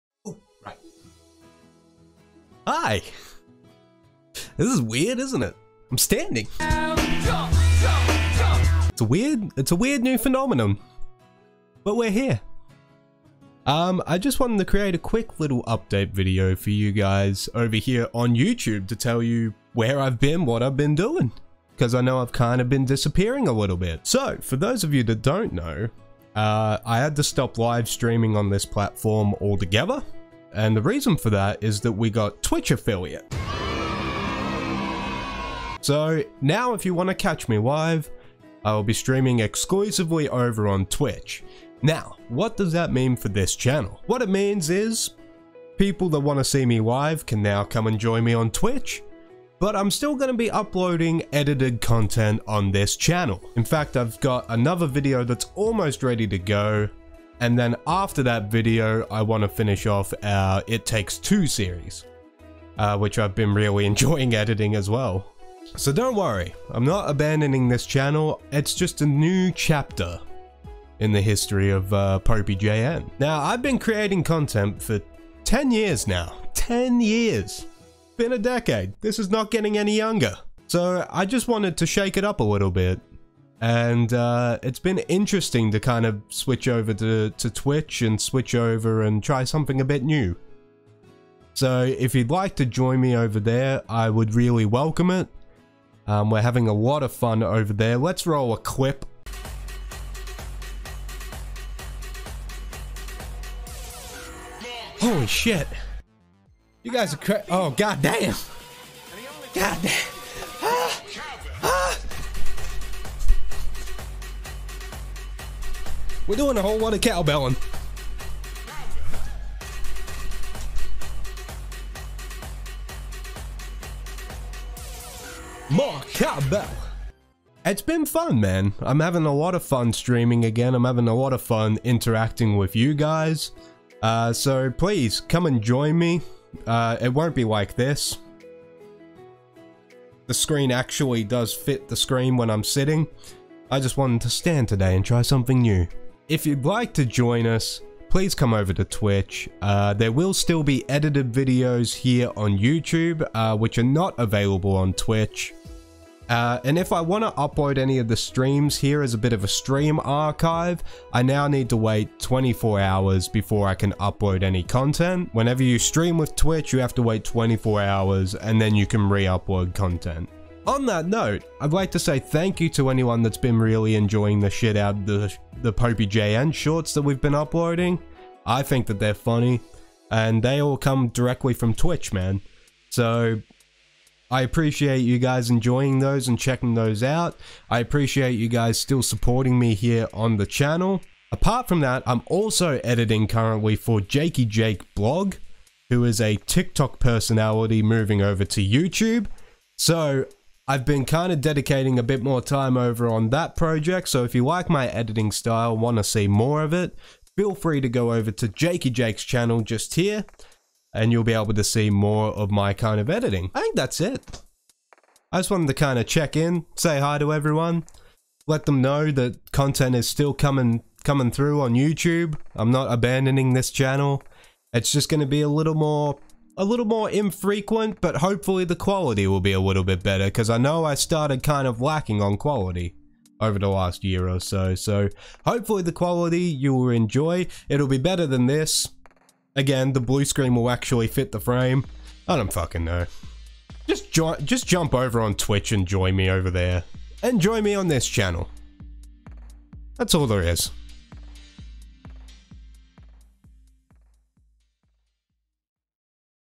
Quick Channel Update
I Know I've Been Inconsistent For A While... Here's An Update.